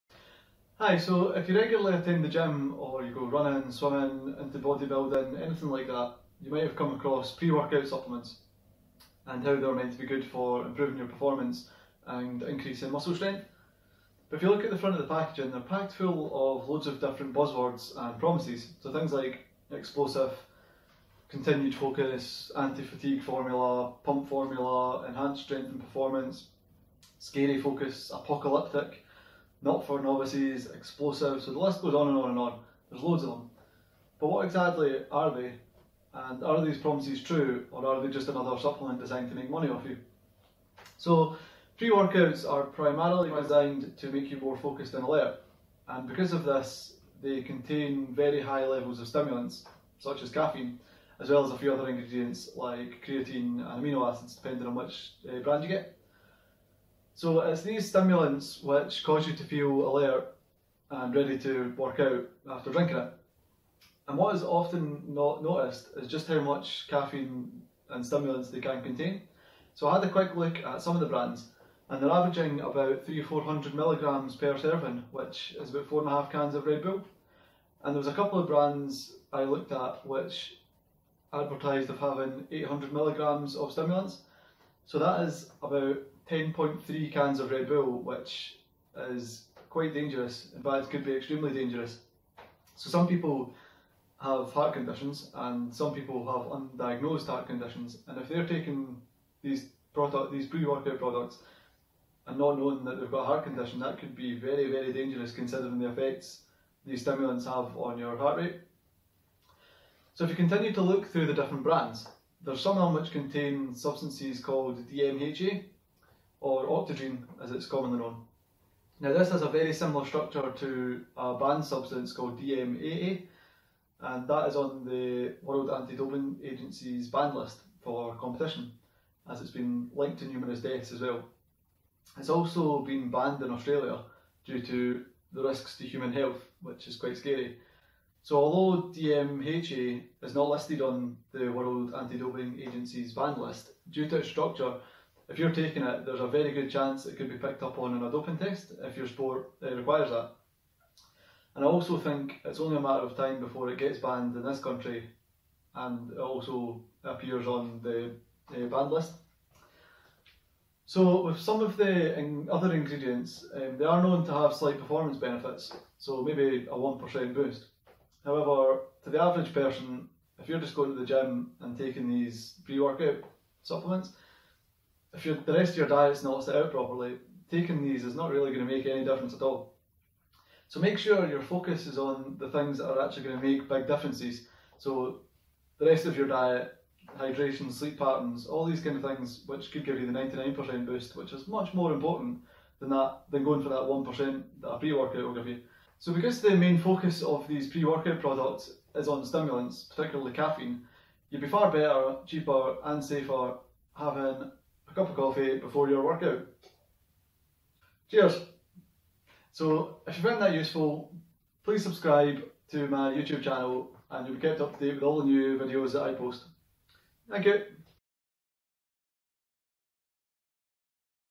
What actually are Pre-Workouts and are they worth it?
Pre Workout supplements are big business at the moment but are they actually worth it? Here is the view of a professional sports nutritionist. Perhaps there are better and safer options than shop bought products.